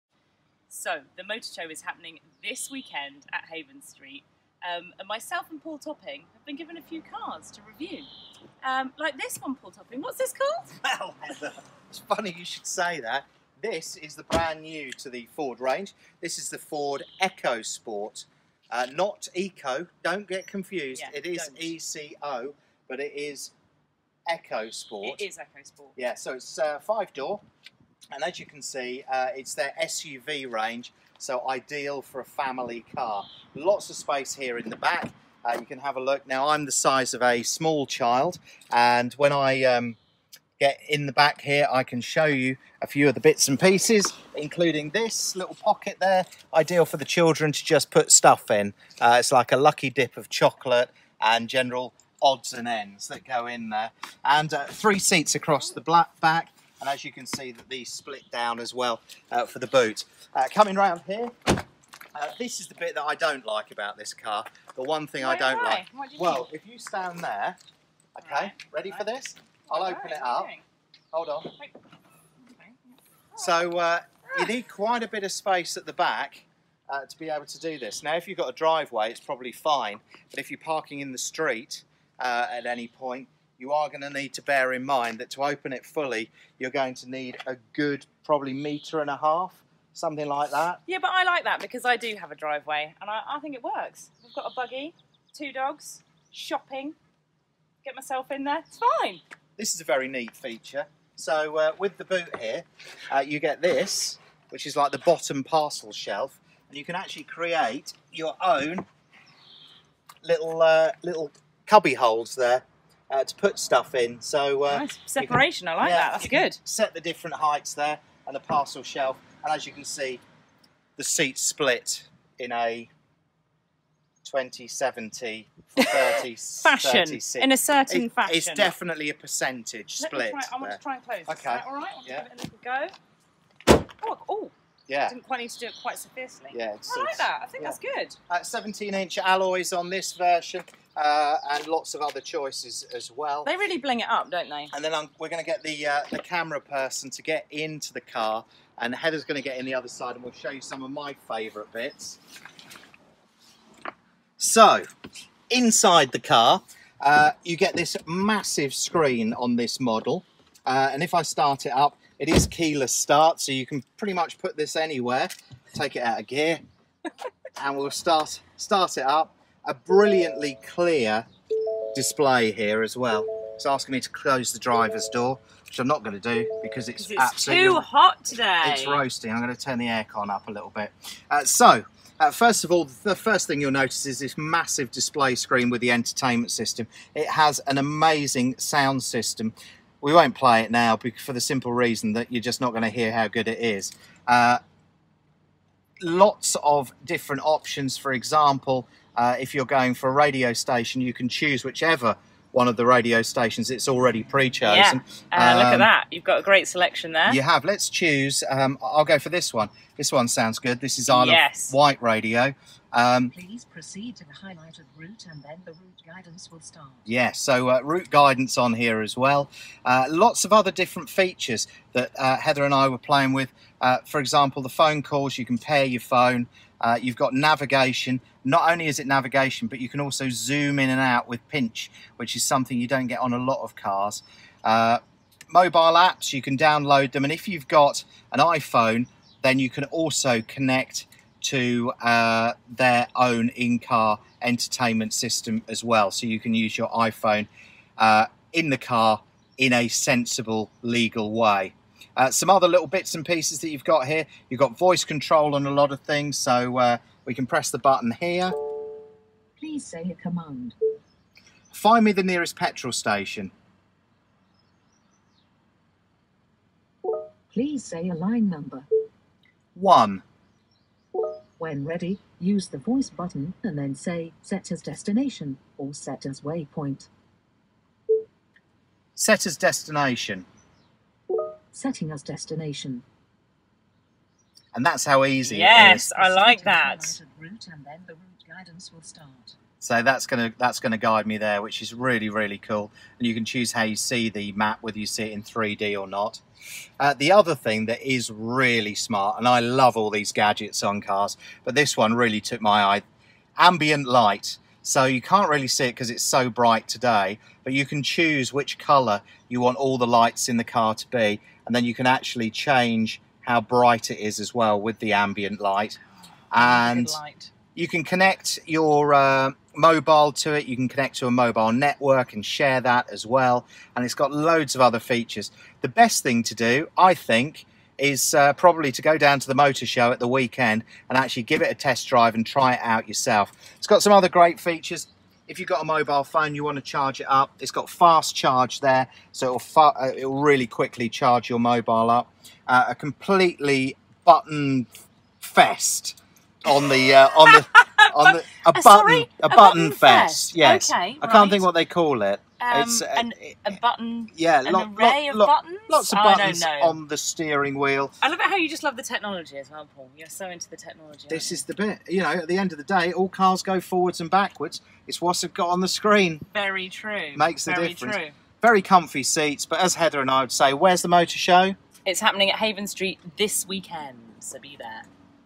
Ford EcoSport Review | Isle of Wight Radio
Paul and Heather take a quick look at the New EcoSport: Rugged Looks In Rugged Territory . EcoSport Range From £17,295 OTR

Model tested New EcoSport ST-Line 1.0 EcoBoost (125 PS) 6-Speed with optional extras-From £20,095 OTR

Recently revised, the New EcoSport is now available with Ford’s Intelligent All Wheel Drive Technology with the new and advanced 1.5-litre EcoBlue diesel engine. Meanwhile across the dynamic range, EcoSport is now available with 12 bold colour and contrasting roof colour options. Behind the wheel, drivers will appreciate Ford’s SYNC 3 touchscreen sitting in an upscale and user-centric interior.

Revised design of EcoSport, featuring a smart new grille and new LED daytime running lights. Highly energy efficient, these advanced daytime running lights help get you noticed during the day and seen clearly at night.
Ford’s smallest car in their SUV range, has rugged design cues and an elevated driving position.
The car is now built in Romania to ensure the EcoSport is a great European car with quality refinement. The seats have been redesigned to be more supportive for drivers and passengers alike.
Behind the rear tailgate door, you’ll see the load floor in the cargo area has three height settings, so you can create a hidden compartment of varying size under the boot floor. When set in its highest position, and with the rear seats folded, you get a flat load floor.
The EcoSport’s utility looks continue inside with the rear seats can folded down to give you an impressive 1,238 litres of space in the back. The seats can be split 60:40, they can fold forward to make room for taller objects and, to help make best use of space.
That utility feel continues with plenty of storage space in the cabin including a handy bin under the headlight switch, a slim open storage bin above the glovebox, a shelved glovebox with a large illuminated bin for drinks cans and other items. That plus a sunglass holder, generously sized side pockets in the doors, coat hooks, boot tie-down loops.
For the first the New EcoSport is available with Intelligent All-Wheel Drive (AWD) system which can determine how much grip, cornering balance and responsiveness is required in wet, dry or icy conditions and on varying terrain. The technology is able to send drive to the front or rear axle depending on requirements. This option is only available with the 1.5 EcoBlue diesel engine.
Quickclear Heated windscreen is now a standard feature throughout the EcoSport range: This clever feature is designed to get you on the move quickly on frosty mornings. Just touch a button and ultra-thin filaments can superheat to de-ice, de-fog and de-mist your windscreen as well as help defrost the wipers.
Electric power-assisted steering (EPAS) automatically adjusts to your speed and the conditions. At low speeds, the system makes wheel turns feel lighter and easier, improving manoeuvrability. At high speeds, when you need greater control, it firms up the steering. Now with drift control, EPAS also takes into account crosswinds and other driving conditions, while active nibble control reduces any unwanted vibration in the steering wheel.
Extra convenient optional extras include: Heated front seats for both driver and passenger and whether it’s a frosty morning, a chilly day, or a freezing cold night, a heated steering wheel is also guaranteed to warm your hands better than a pair of gloves.